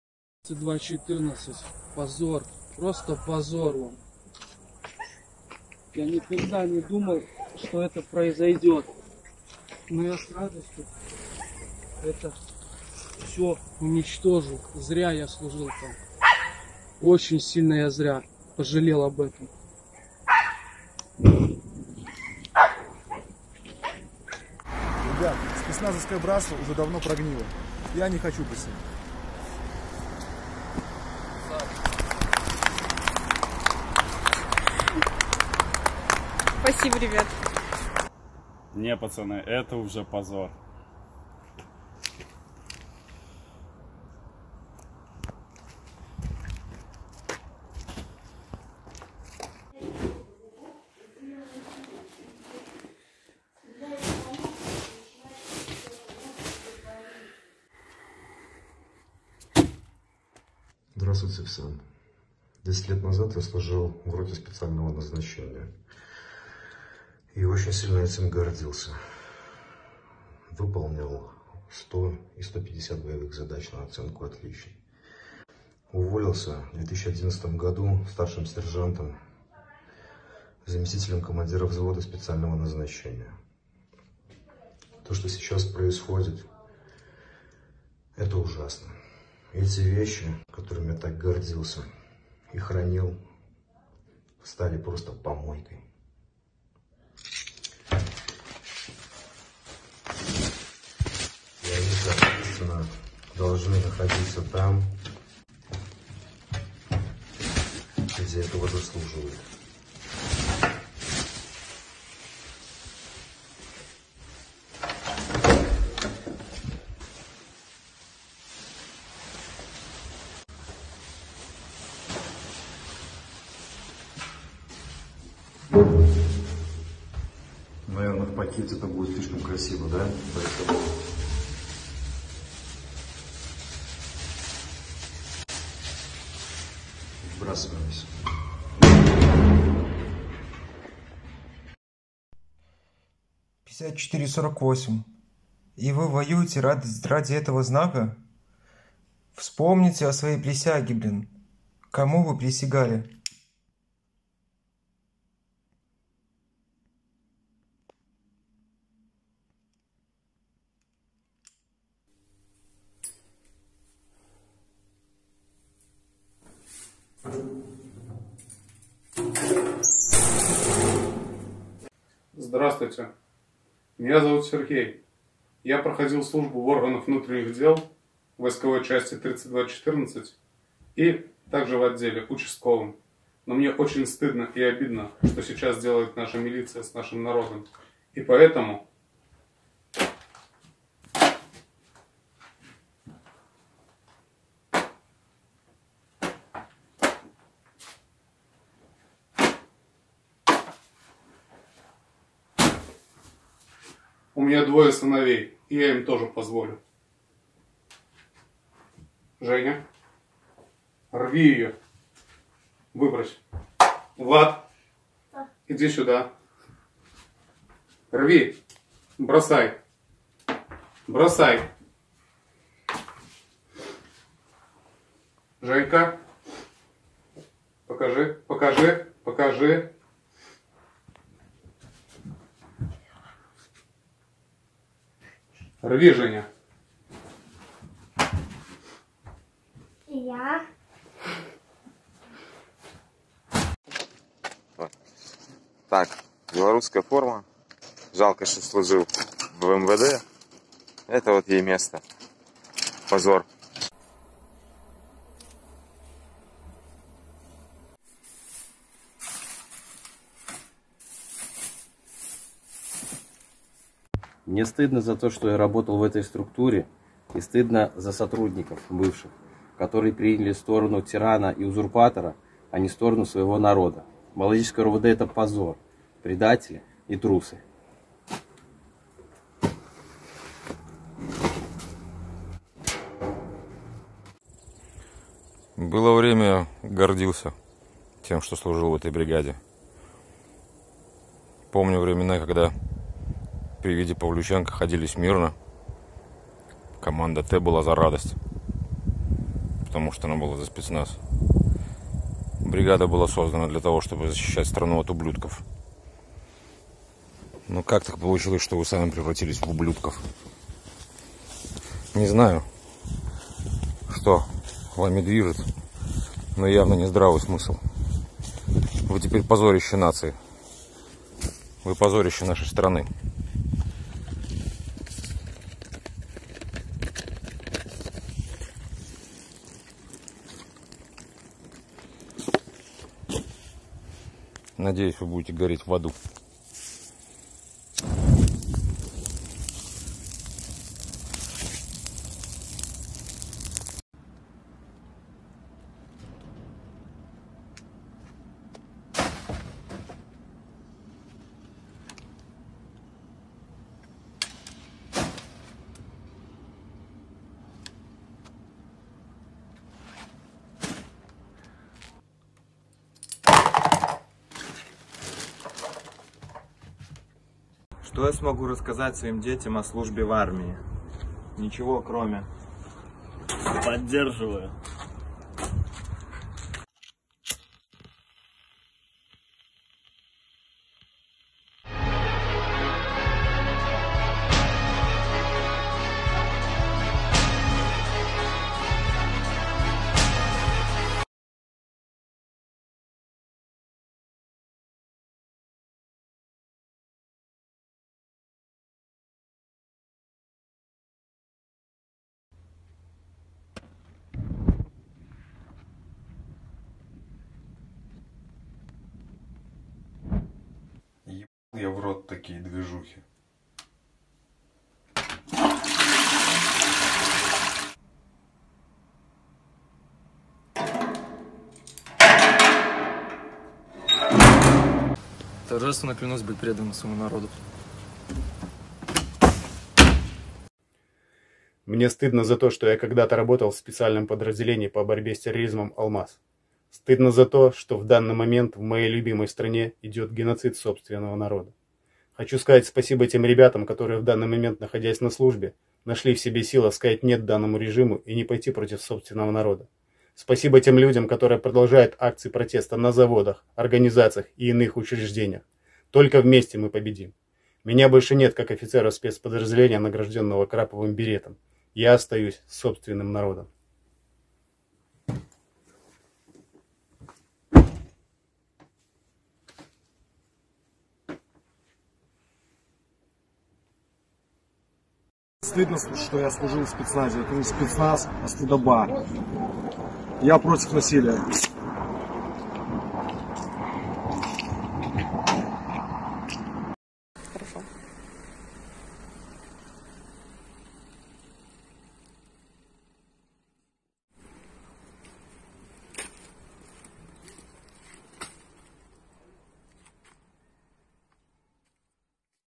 Силовики сжигают и выбрасывают форму
Август 2020, Беларусь. Бывшие офицеры спецназа и сотрудники милиции выбрасывают и сжигают свою форму и знаки отличия, а также призывают коллег не исполнять преступные приказы.